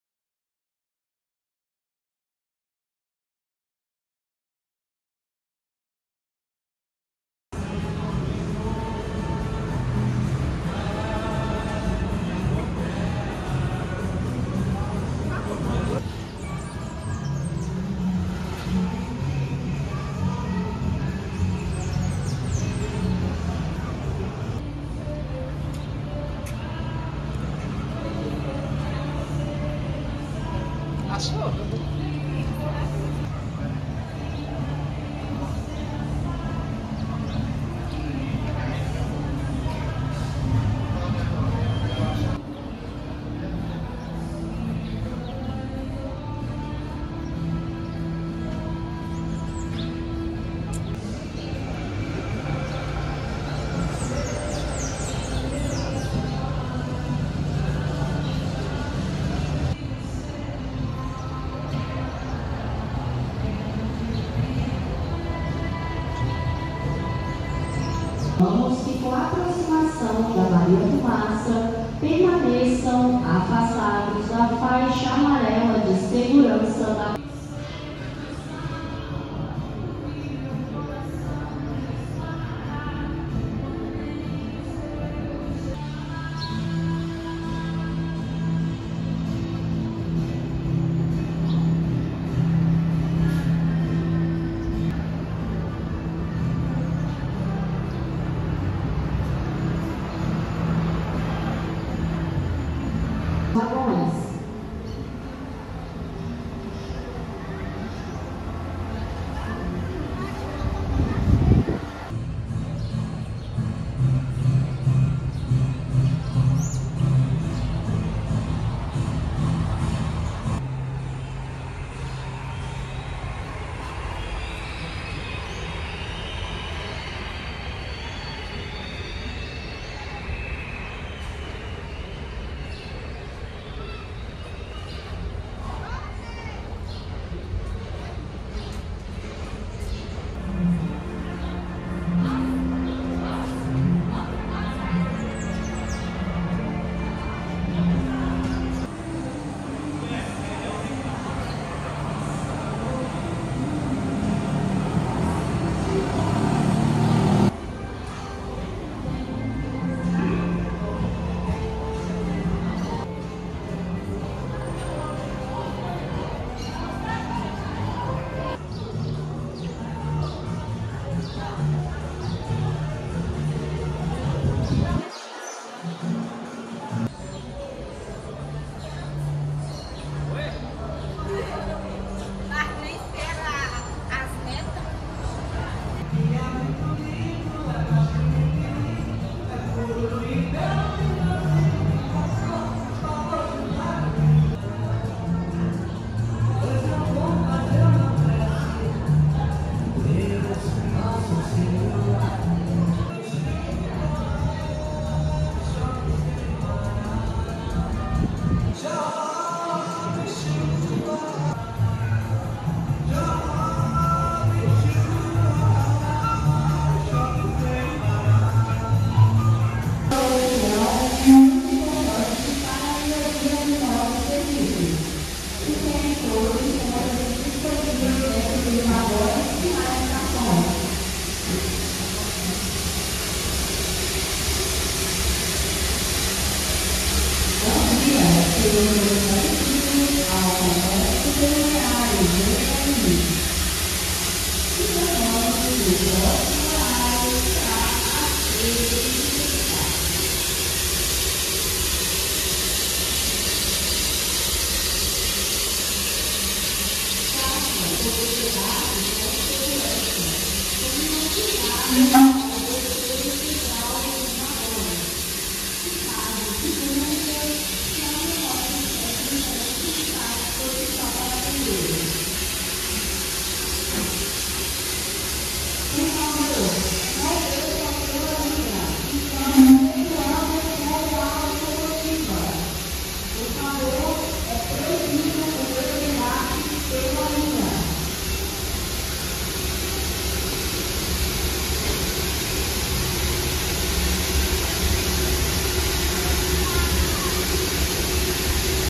Locomotiva 22 - E.F.O.M. (Estação ferroviária de São João Del Rei/MG).
O passeio da maria fumaça liga as duas cidades históricas, São João Del Rei e Tiradentes. O percurso completo tem 12KM, e a viagem tem a duração de 45 minutos.

Tem interesse em realizar o passeio? Se sim, acessem este link e garanta sua viagem com antecedência:

Locomotiva 22:
Fabricante: The Baldwin Locomotive Works

Data de fabricação: 07/1908

Nº de série: 32878

Classificação White: 4-4-0 American

Longerão: outside-frame (externo às rodas, com contra-balanços

Bitola: 0,76m

Primeiro nº: 22
Segundo nº: 19
Atual número (original)nº: 22